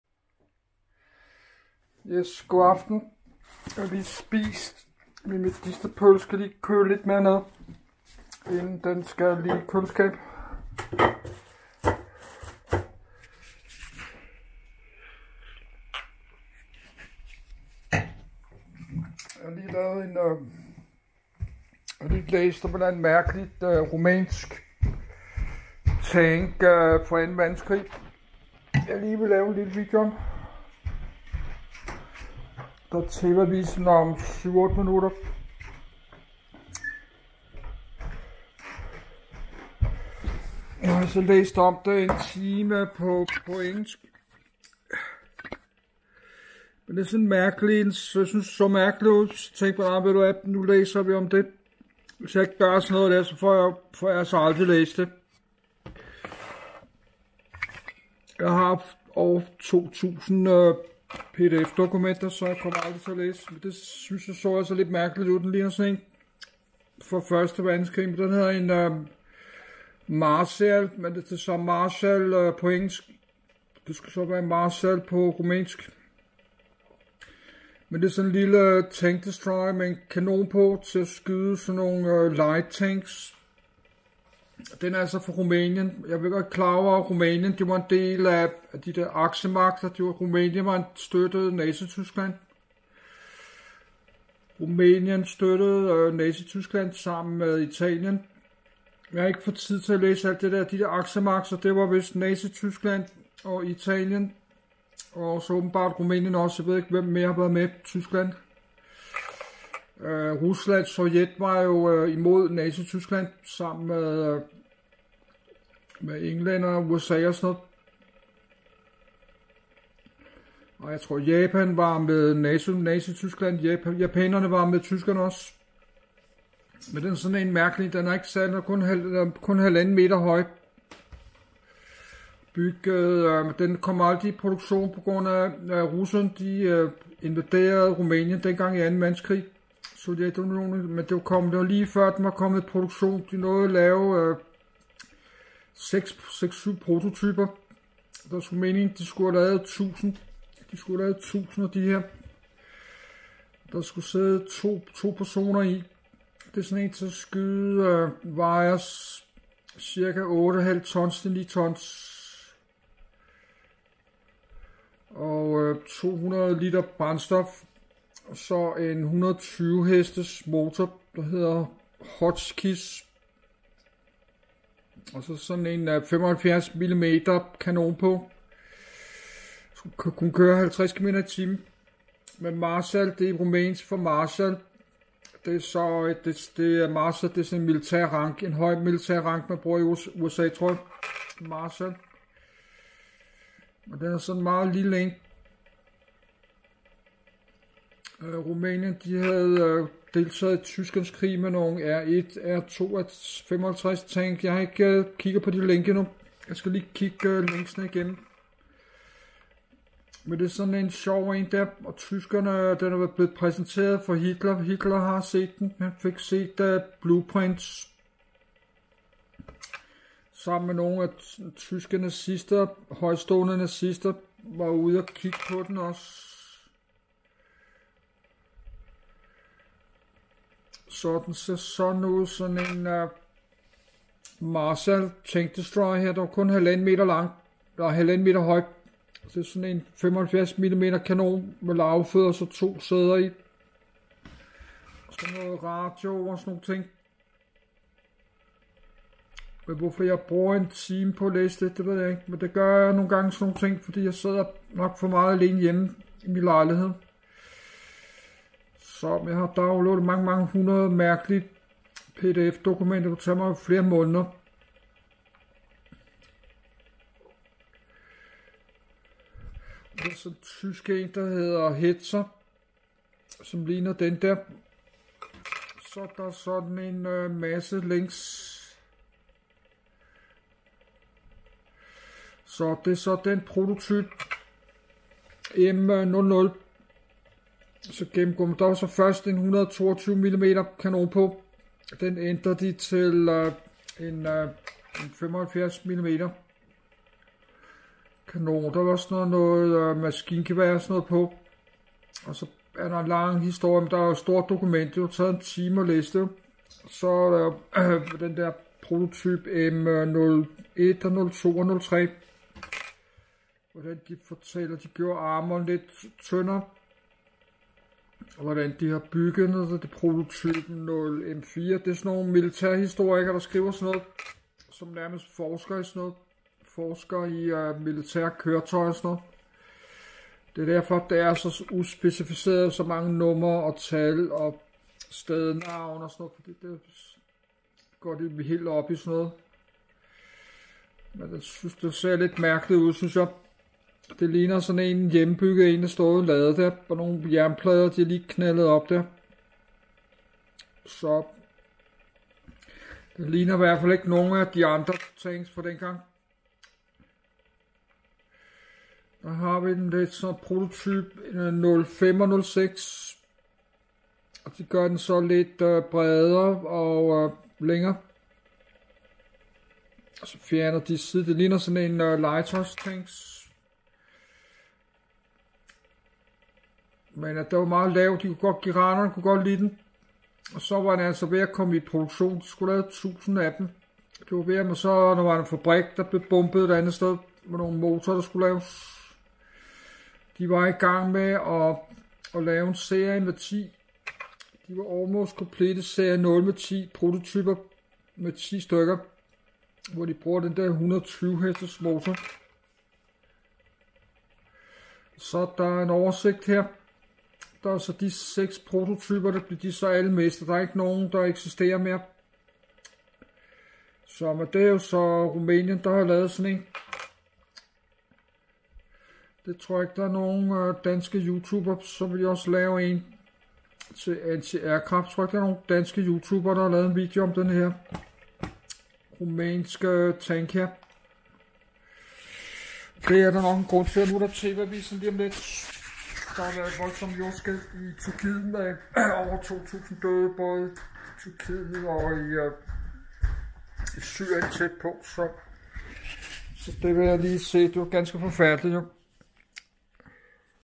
MongoTV_11500 - KAMPVOGN - TANK - Del 27 - Maresal - Tank Destroyer - WW2 - Rumænien
DEL 27 - God MANDAG AFTEN. Jeg syntes, at den her gamle rumænske tank så lidt underlig ud, og fik lyst til at læse om den på engelsk ? :-) Det her blev en lidt kort video, da jeg er træt nu. Jeg ville gerne se TV-Avisen klokken 18.30. Så det gik lidt hurtigt. Klokken den er lidt over 19 nu. Godaften.